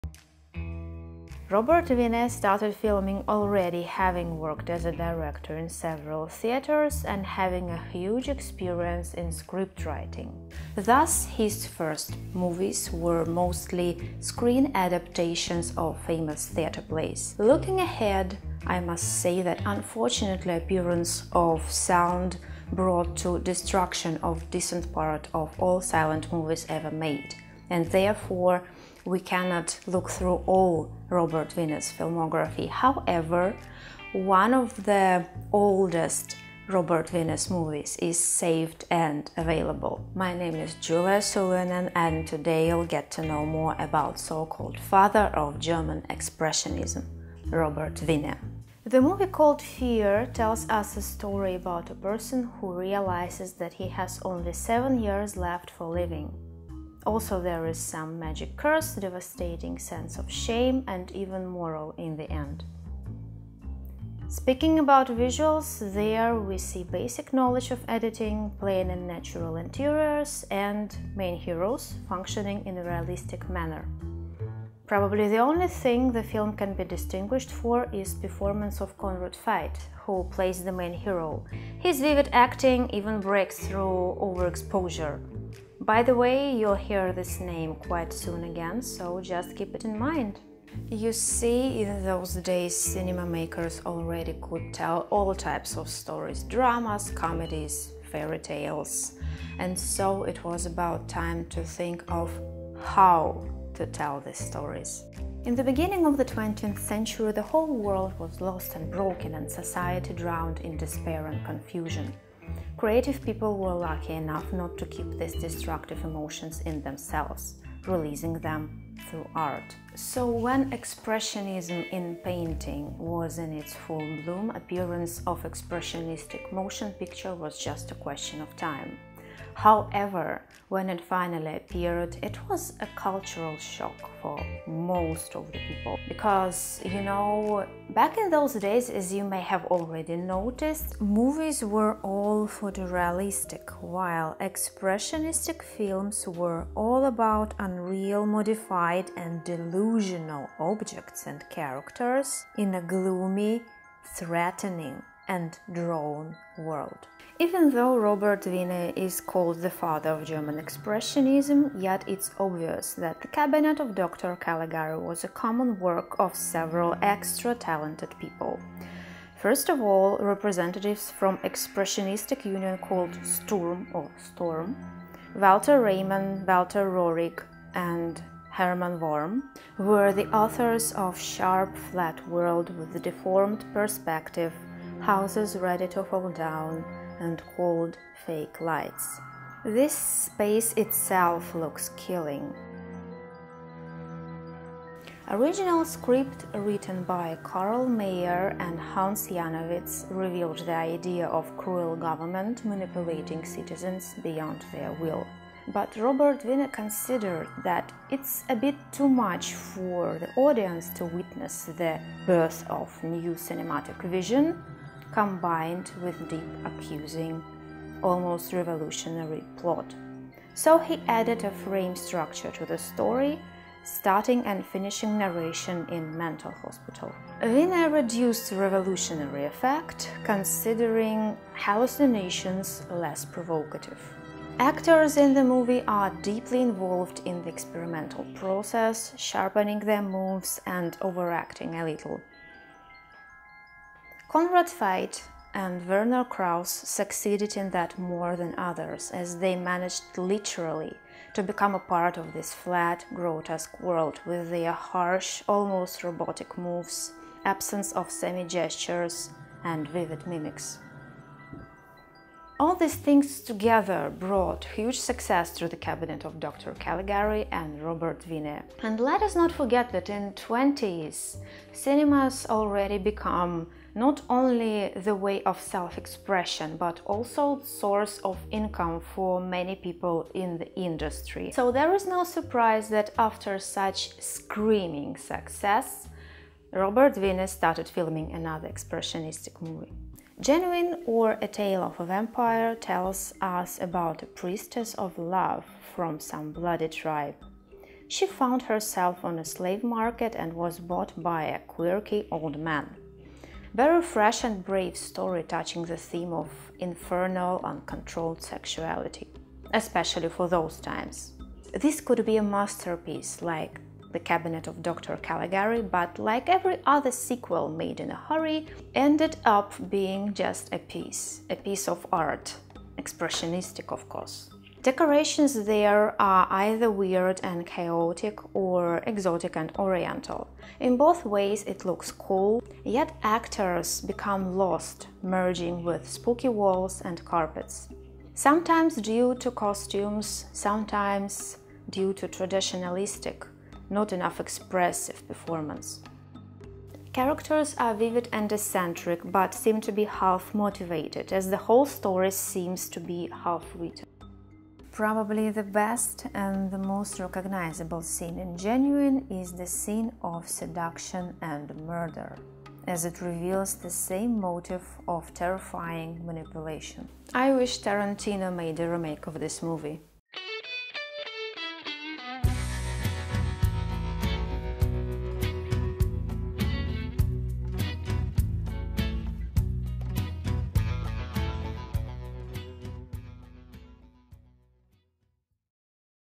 Silent Film History: Robert Wiene (German Expressionism, The Cabinet of Dr.Caligari, Genuine)
Here I'd like to start a thrilling story about the greatest creators of the silent era in film history. And the first representative is Robert Wiene, the father of German expressionism. You'll come to know about his most famous films and how his works were changing throughout the years.
In the video you'll also find several works of famous expressionists: Hubert Roestenburg, Ernst Ludwig Kirchner, Otto Dix, Alexey Javlensky and Ludwig Meidner